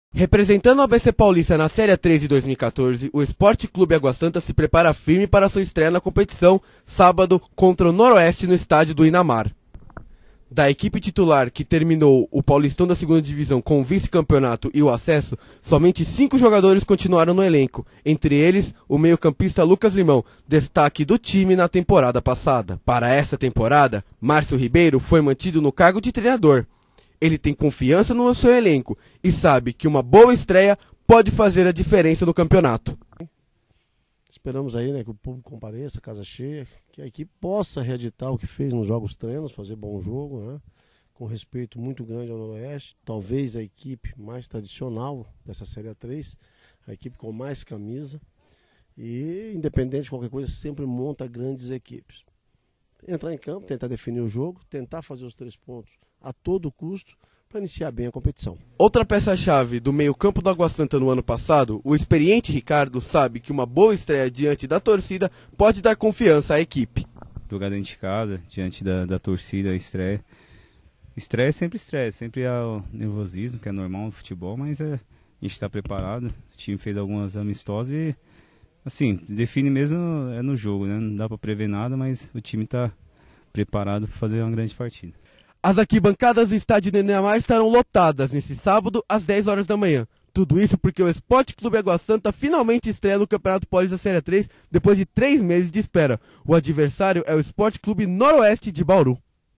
Último treino do Água Santa antes da estreia na Serie A-3
Último dia de treinamento do Esporte Clube Água Santa antes da primeira partida do clube no Campeonato Paulista da Serie A-3